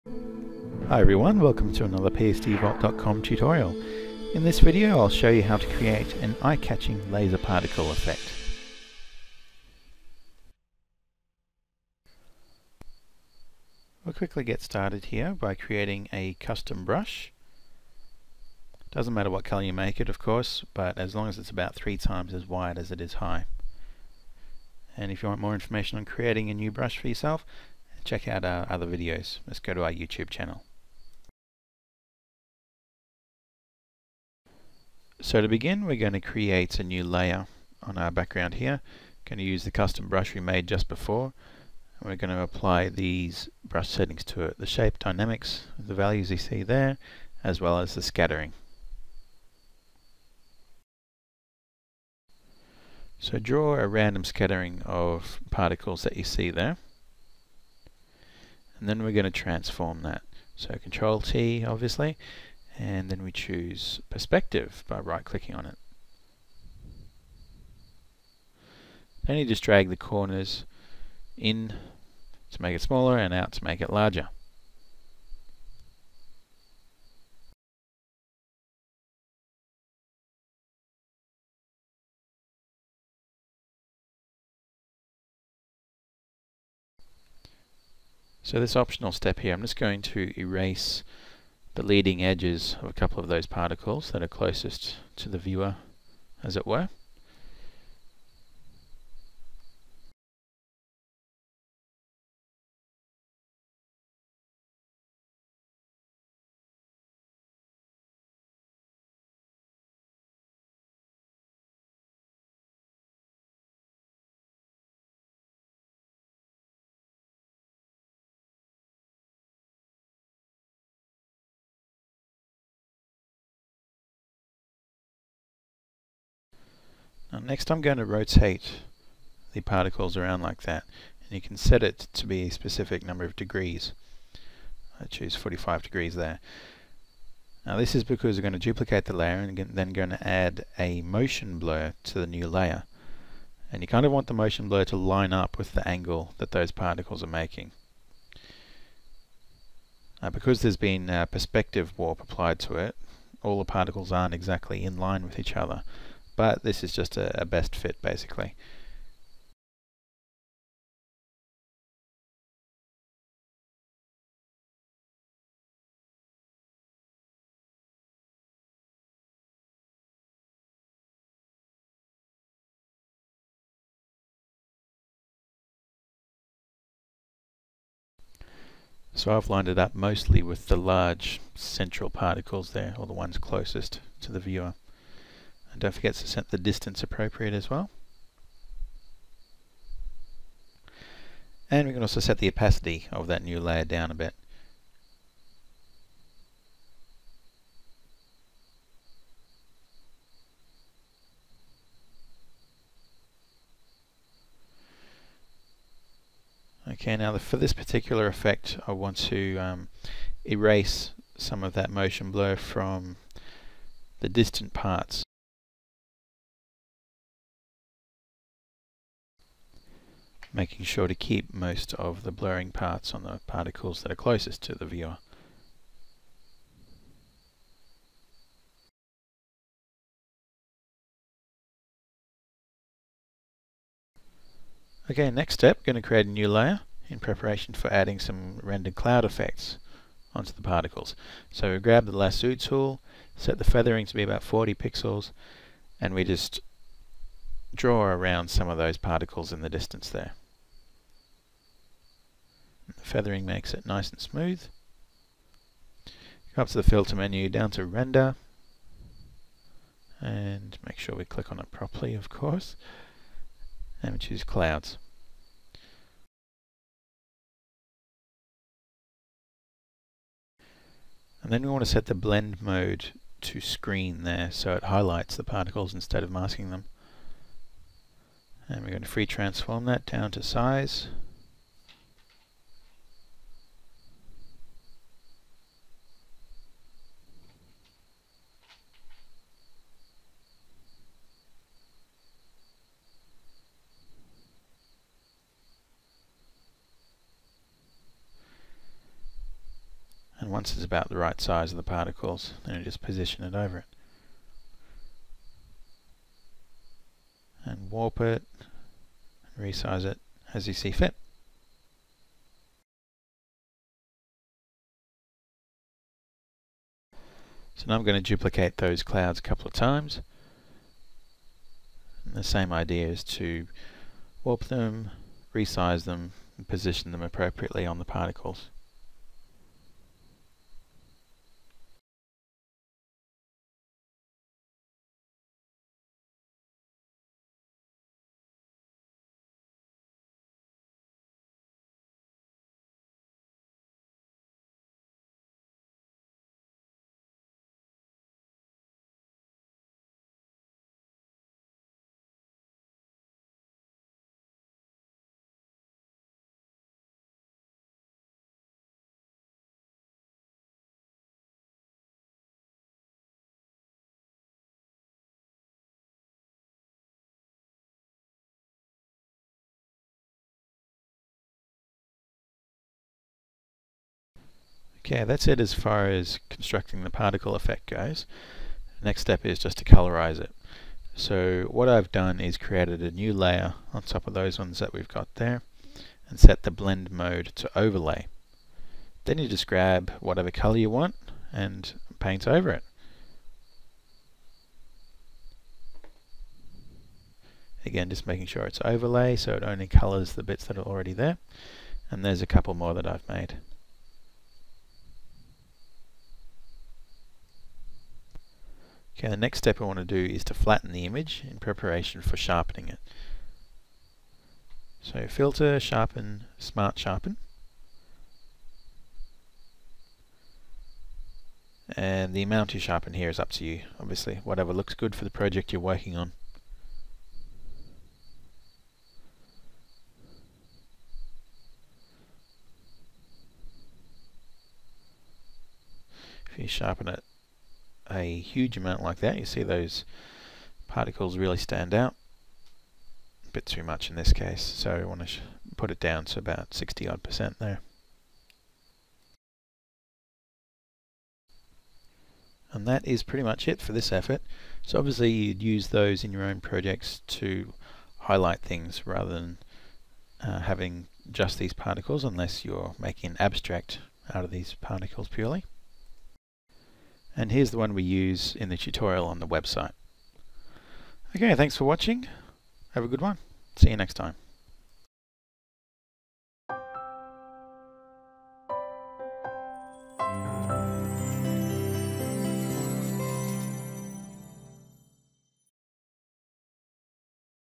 Photoshop Tutorial - Create Attractive Laser Effect in Photoshop
In this tutorial, I will show you how easy it is to design this very eye-catching, Laser Particle abstract effect in Photoshop.

If you like the video please rate, comment, or add to your favourites. It's really easy and will help us a lot :)